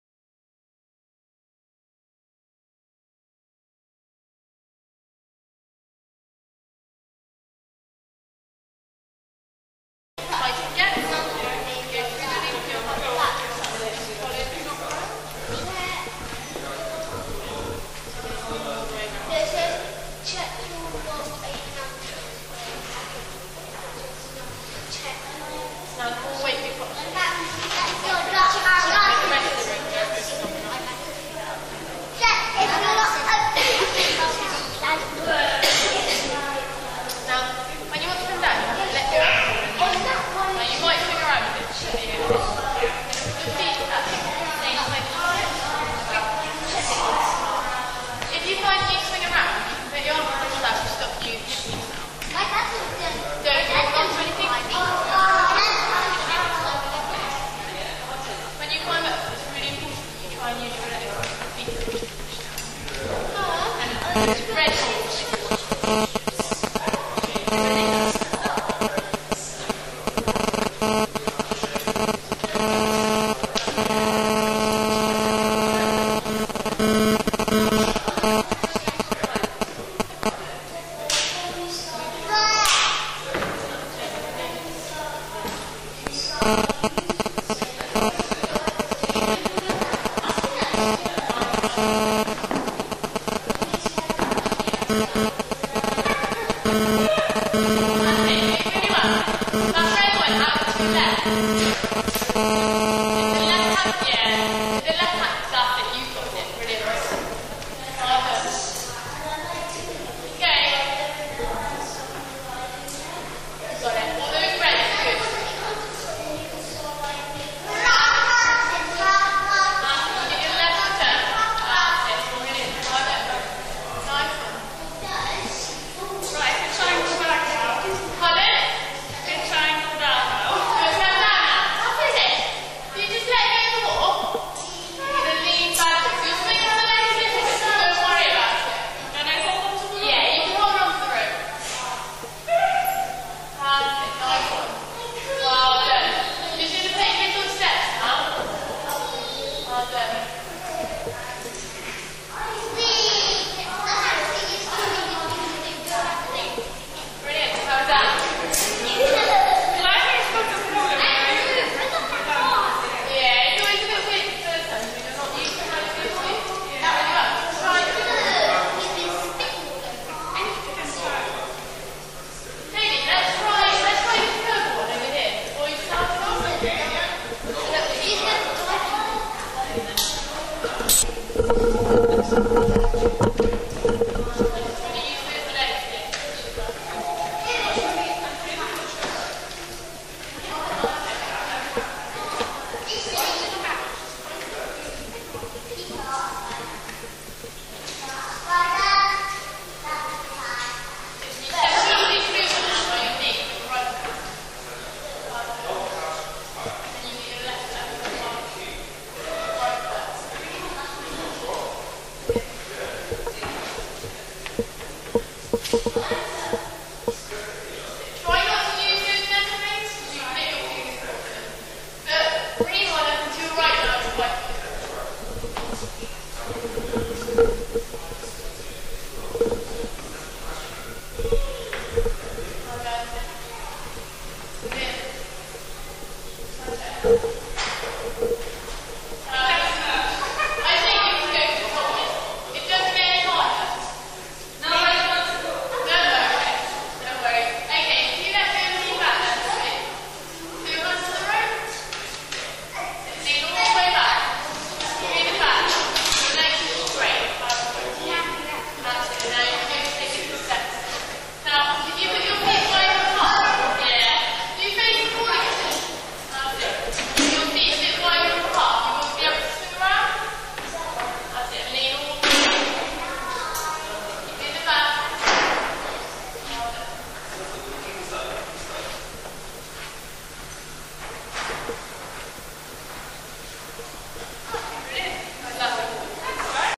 Mum - first time climbing - the Warehouse, Gloucester, UK
Family Fun at The Warehouse - Climbing Wall.

This was great - boys aged 7 and Mum all had fun climbing the Wall and bouldering.

The Warehouse is a really good resource for Gloucester, have a go, you'll enjoy it.